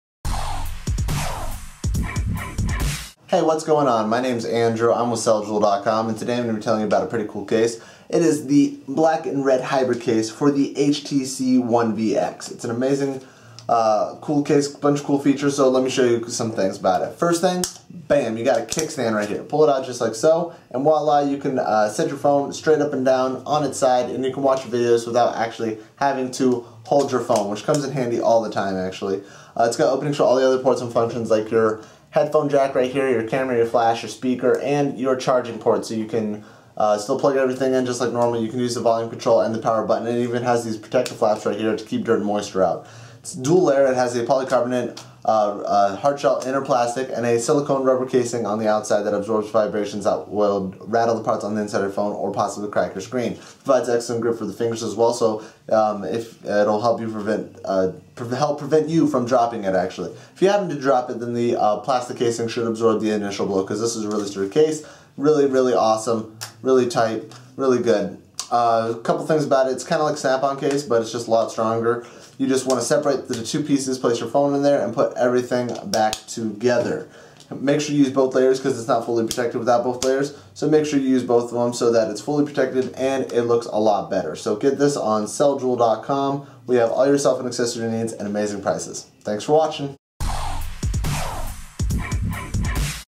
CellJewel.com - HTC One VX Black Skin/Red KickStand Hybrid Case
Ultimate protection for absorbing shock from bumps and drops!

Form-fitting Protection for Bumps and Drops
This rugged two-piece case is designed specifically for your phone or tablet. It combines the durability of a plastic inner case and the flexibility of a rubber outer case to give you maximum shock-absorbing protection.

Non-Slip Comfortable Design
The outer rubber skin provides easy, comfortable handling and prevents skidding and slipping.

Complete Access to Ports and Functions
Precise button placement and cut-outs allow you to use all of your phone's functions. Take pictures, adjust volume, and connect your charger with ease.

Easy Hassle-Free Installation
There are no tools required to put this case on! Simply snap the plastic case onto your phone and then stretch the rubber skin over that for a secure, perfect fit.